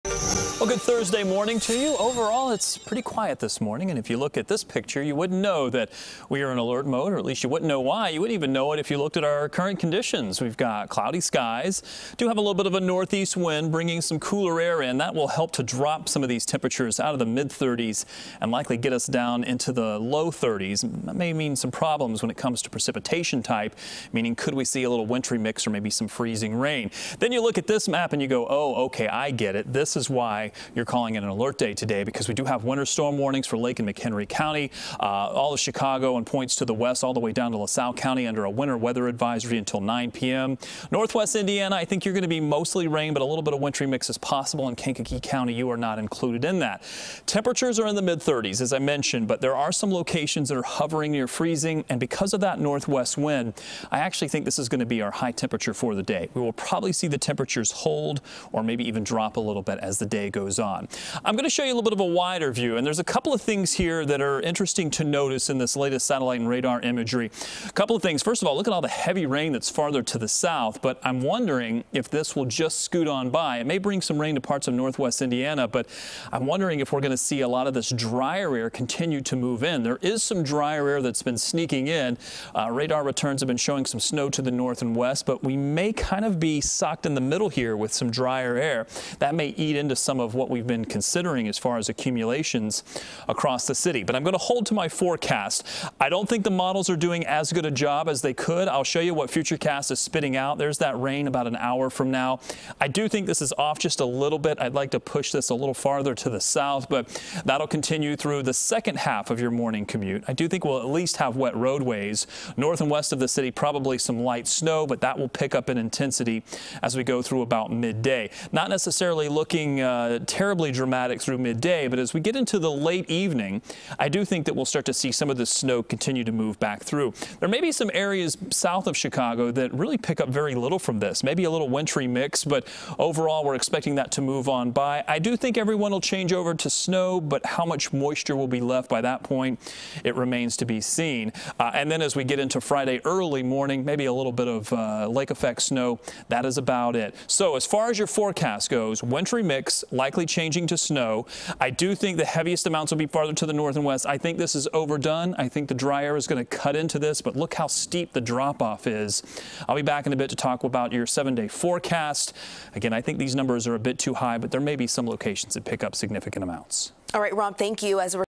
Chicago Weather Alert: Winter Weather Advisory in effect, 2 to 4 inches of snow expected
CBS 2 meteorologist Robb Ellis has the forecast.